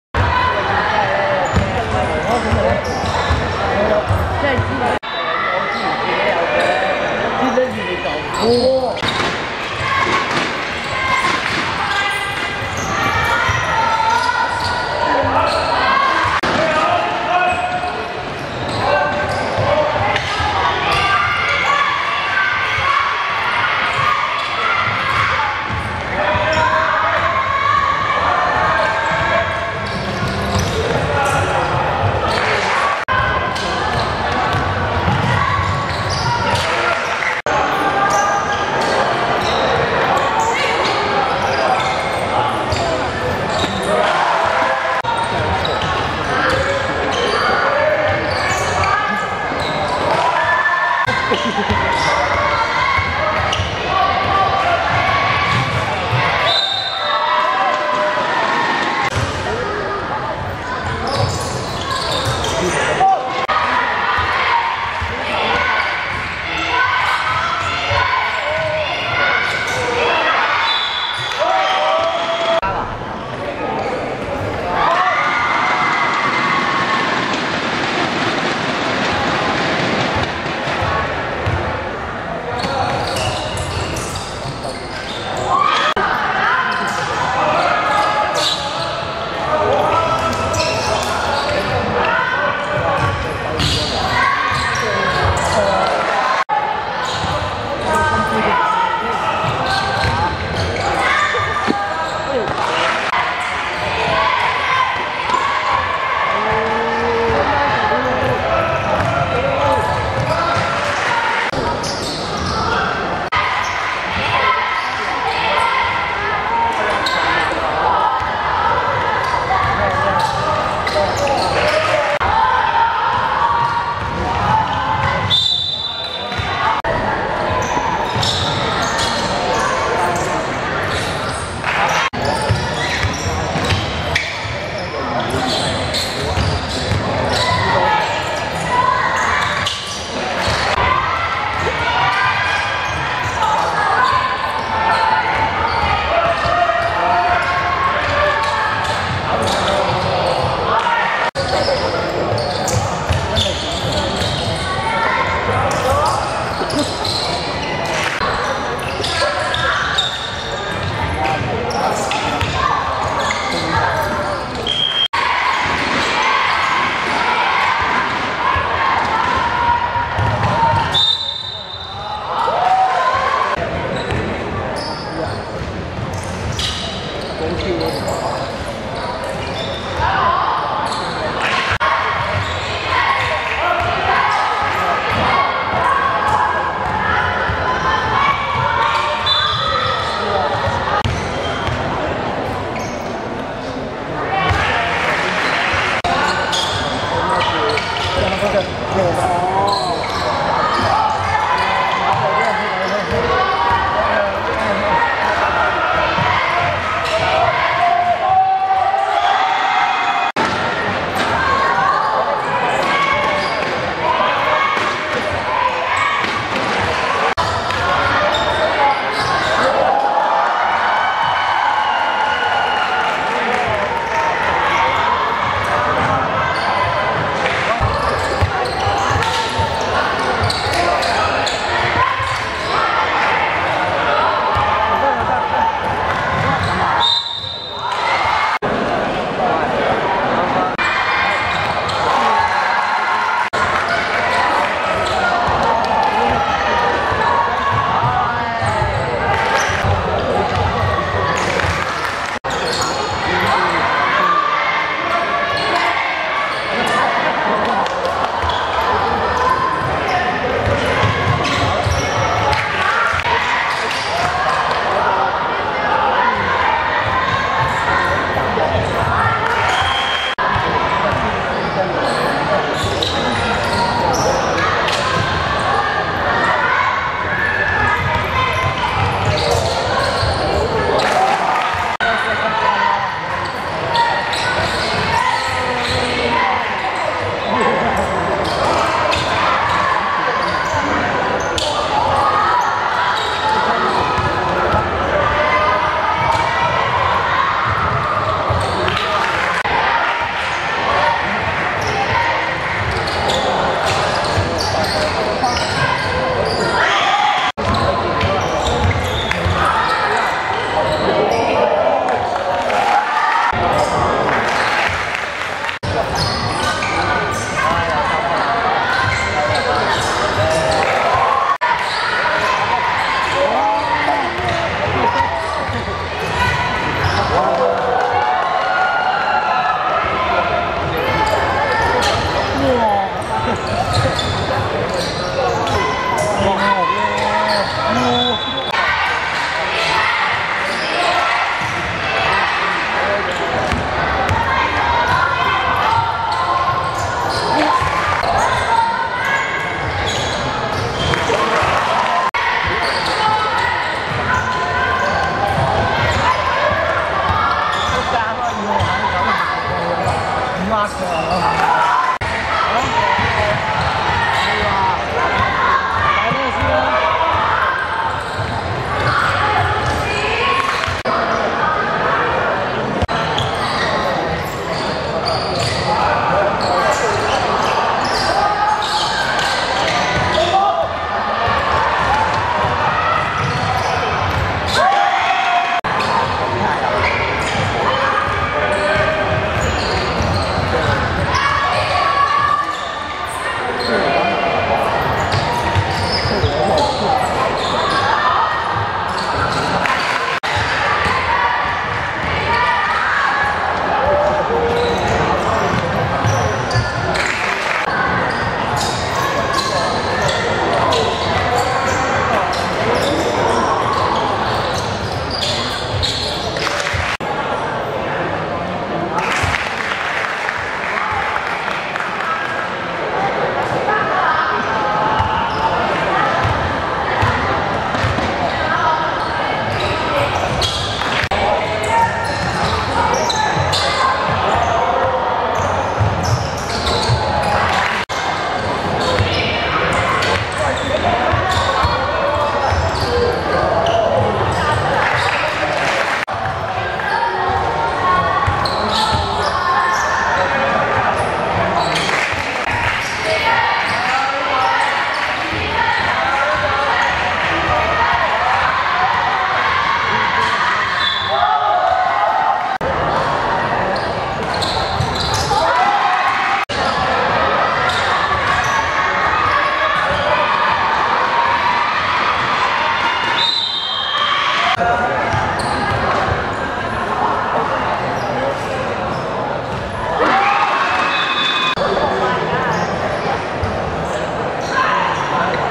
天循vs青年會(2016.4.12.元朗學界女子籃球乙組決賽)精華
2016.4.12.天瑞體育館 17:00 天循(白) 11 : 90 青年會(黑)